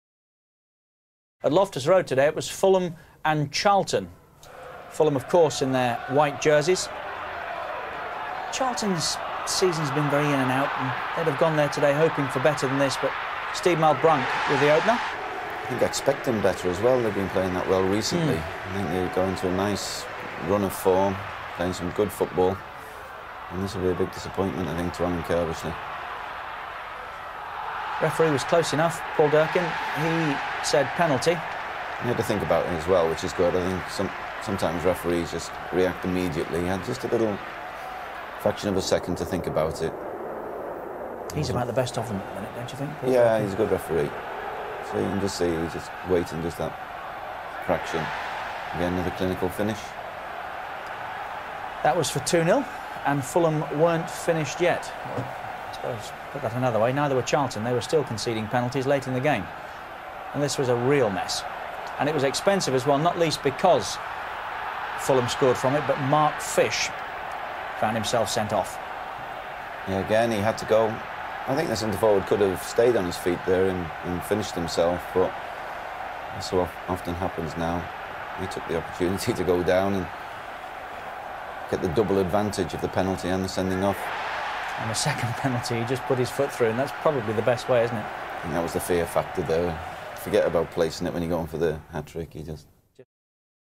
2002/03 Fulham v Charlton Athletic FAC 4th (Highlights)
26th January 2003 (Sun. – 2 p.m.) at Loftus Road (12,203) - 4th Round
Fulham 3 (0) (Malbranque 59, 66 (pen), 87 (pen)): Taylor; Djetou (Knight 85), Melville, Goma, Harley; Malbranque (Goldbaek 88), Legwinski, Davis, Boa Morte; Sava, Marlet. Subs. (not used) Herrera, Inamoto.
Charlton Athletic 0 (0): Kiely; Young (Fortune 72), Rufus, Fish, Konchesky; Kishishev, Parker (Svensson 78), Euell, Jensen (Bart-Williams 72); Johansson, Bartlett. Subs. (not used) Rachubka, Powell.
Referee: Paul A. Durkin (Dorset).
Sent off: Mark Fish (87) for a ‘professional’ foul on Sava – leading to Fulham’s second penalty.
Booked: Jensen (24), foul on Malbranque; Young (27), foul on Boa Morte; Konchesky (81), foul on Malbranque.
Penalties: Young (66), foul on Boa Morte; Fish (87), pulling back Sava – which led to the sending off of Fish.